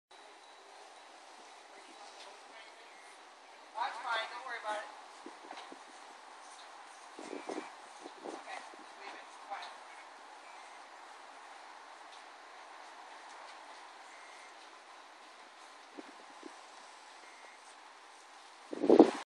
Winter Wonder Yard 2014
Hi everyone, some of you must have remembered our Winter Wonder Yard 3 years ago after the great blizzard here in Chicago in 2011. This time at 2014 we have it again. This is our Winter Wonder Yard 2014, also in honor of the 2014 Sochi Winter Olympics. Thanks to Kyle our neighbor who did much of the work.